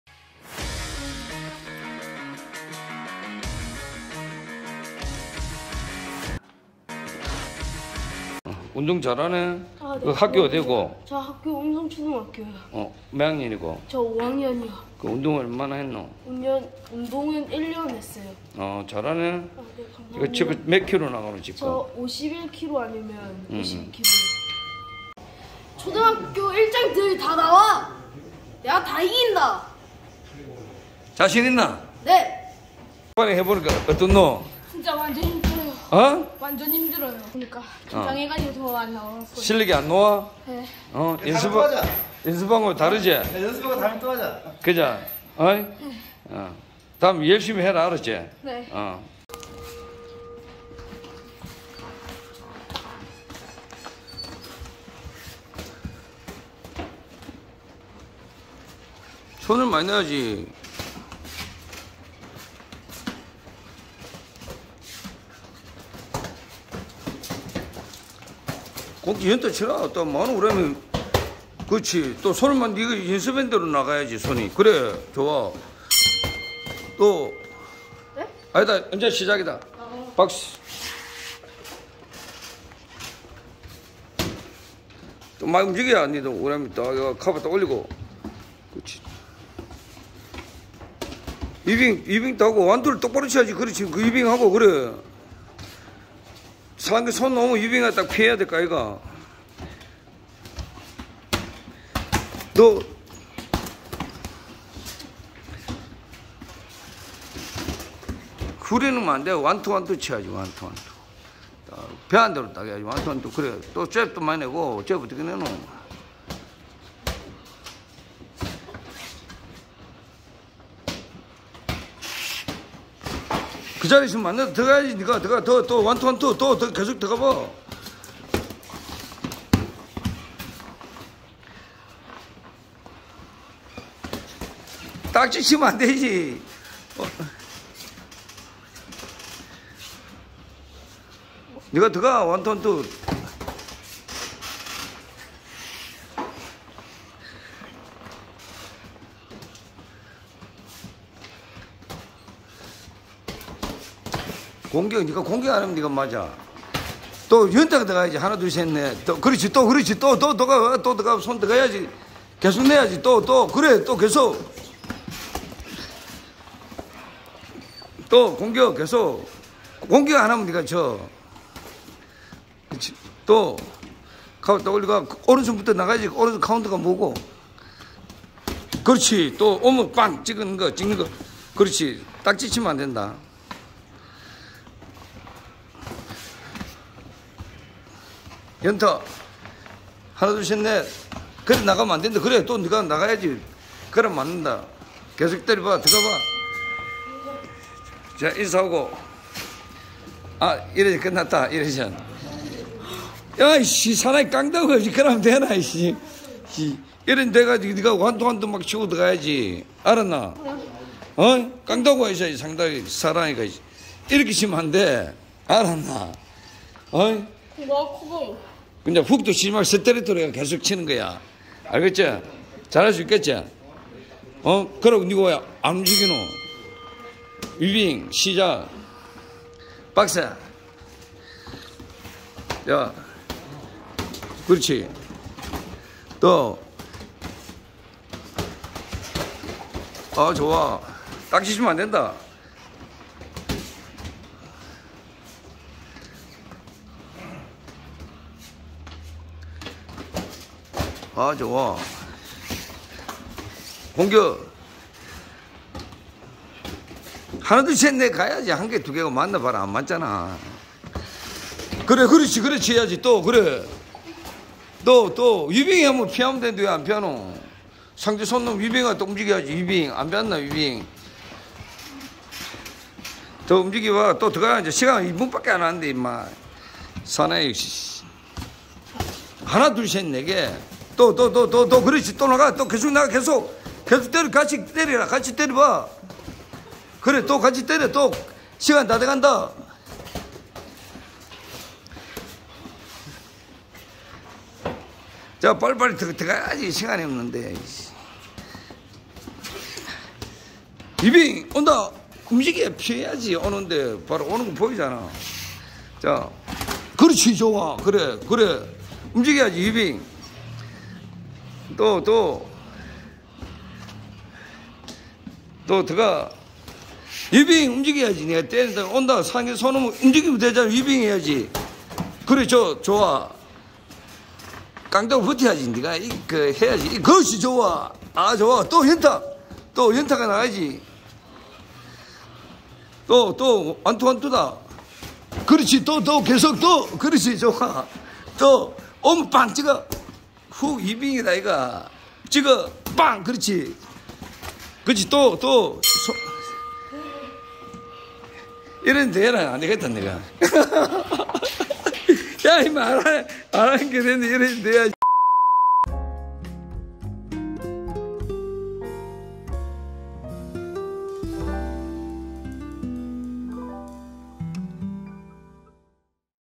많이 맞았는데 신기하게 기분이 좋네요(초등 스파링)
우람이의 첫 스파링에서 많이 맞았지만 최선을 다한 스파링